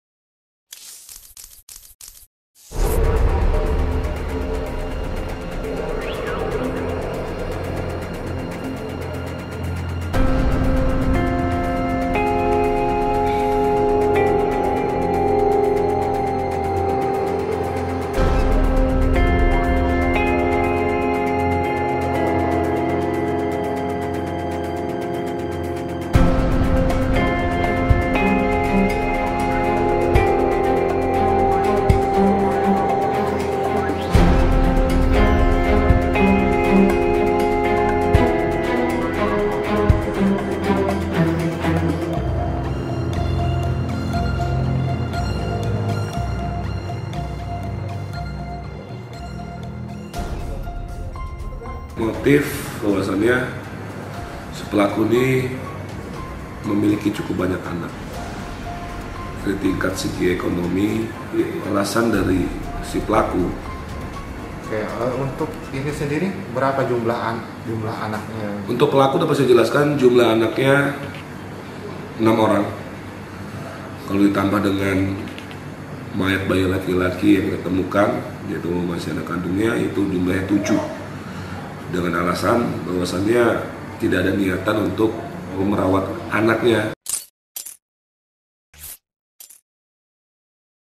Tak Sanggup Biayai, Ibu Ini Tega Buang Anak ke 7 yang Masih Bayi hingga Meninggal
Pelaku pembuang bayi laki-laki hingga meninggal dunia di Bendungan Batu, Kabupaten Tanggamus, Lampung berhasil ditangkap. Diketahui pelaku merupakan ibu kandungnya sendiri.

Kasat Reskrim Polres Tanggamus mengatakan tersangka membuang bayinya karena faktor kesulitan ekonomi. Pelaku mengaku memiliki 6 orang anak dan tidak sanggup lagi menghidupi anak ketujuhnya sehingga nekat membuangnya ke dalam Bendungan.